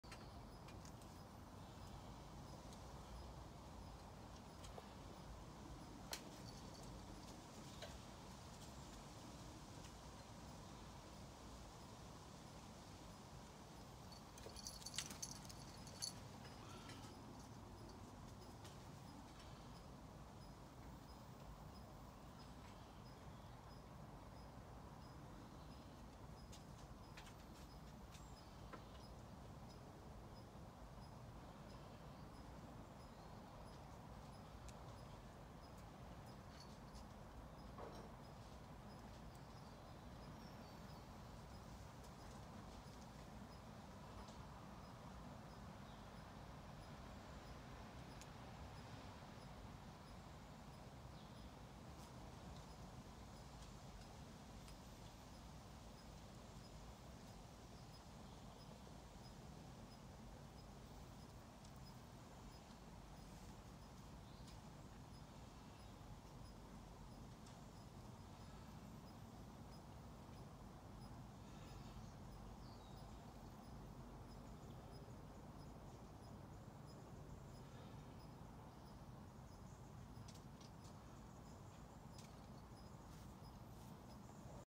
'One small step for kittens"
Two six month old female kittens Molly [black] and Misty [grey] very first steps outside in the big wide world. They were both spayed two weeks earlier, hence the shaved fur. Molly is a Ragdoll-cross and happy-go-lucky whereas Misty, a blue Persian-cross, is skittish and always very cautious.
They grew up together from being ten weeks old.